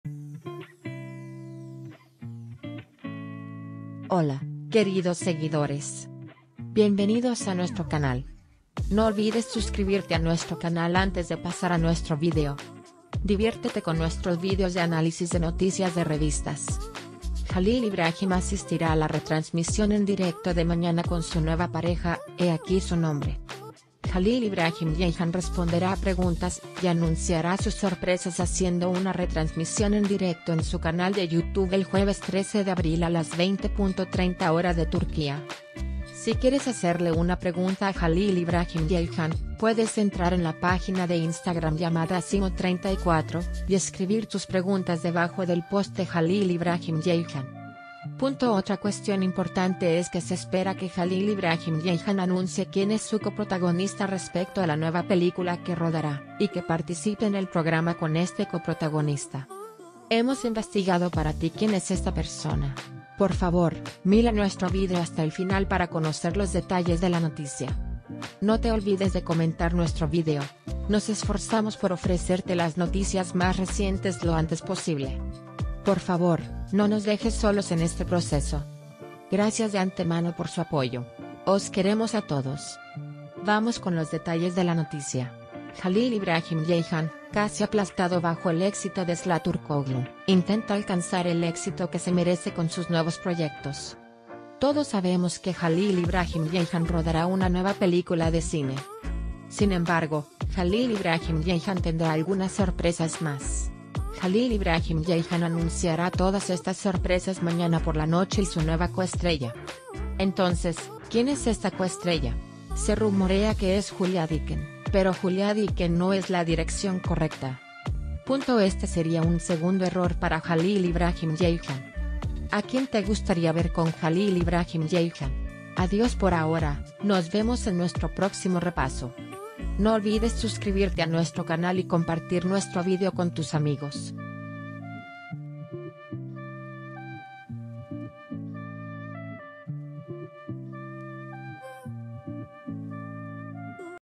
Halil İbrahim se une a la transmisión en vivo mañana con su nuevo compañero.
Halil Ibrahim asistirá a la retransmisión en directo de mañana con su nueva pareja, he aquí su nombre.
Halil İbrahim Ceyhan responderá a preguntas y anunciará sus sorpresas haciendo una retransmisión en directo en su canal de YouTube el jueves 13 de abril a las 20.30 hora de Turquía.
Si quieres hacerle una pregunta a Halil İbrahim Ceyhan, puedes entrar en la página de Instagram llamada Asimo 34 y escribir tus preguntas debajo del post de HALİL İbrahim Ceyhan.